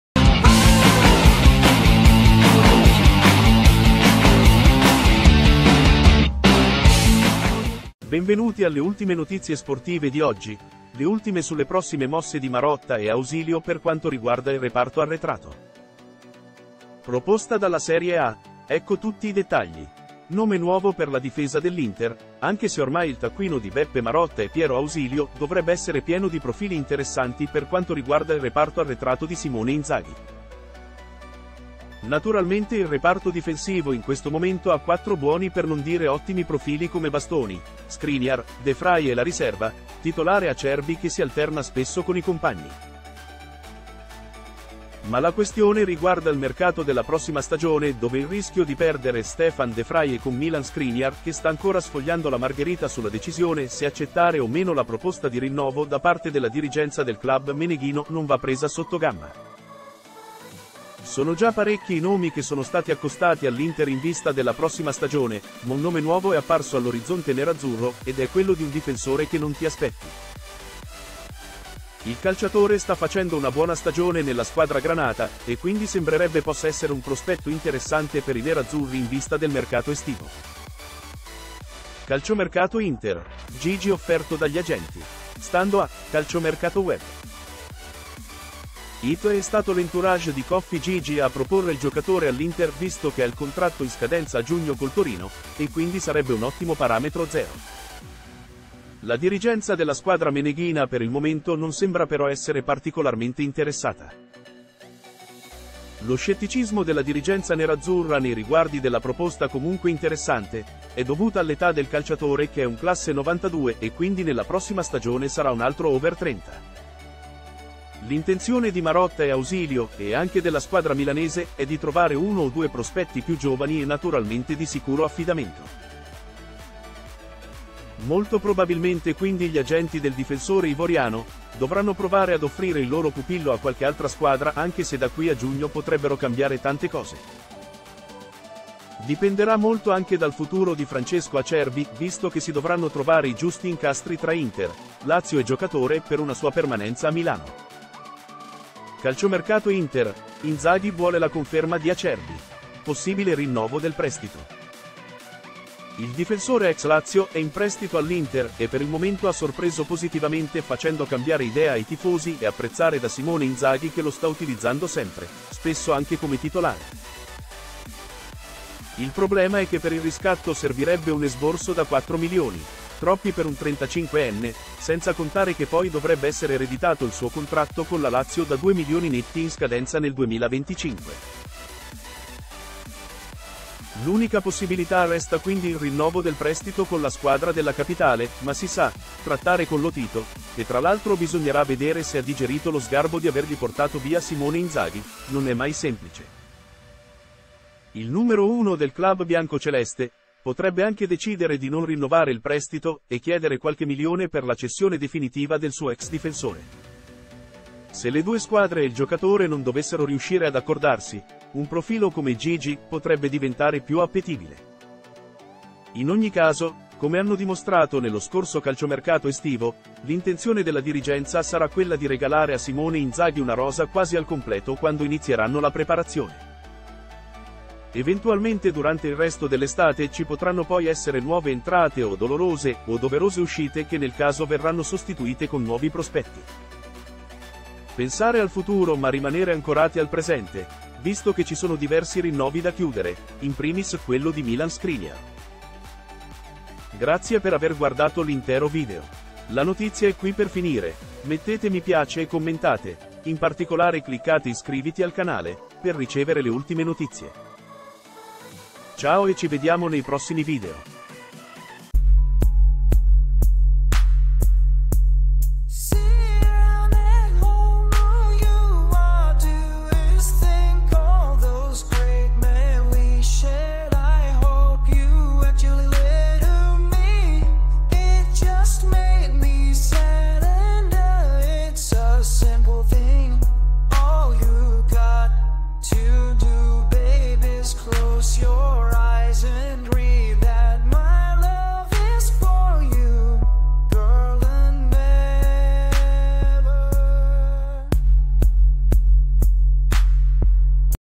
Calciomercato Inter, difensore a costo zero scelta fatta
Le ultime sulle prossime mosse di Marotta e Ausilio per quanto riguarda il reparto arretrato. Proposta dalla Serie A, ecco tutti i dettagli

Nome nuovo per la difesa dell’Inter,  anche se ormai il taccuino di Beppe Marotta e Piero Ausilio dovrebbe essere pieno di profili interessanti per quanto riguarda il reparto arretrato di Simone Inzaghi.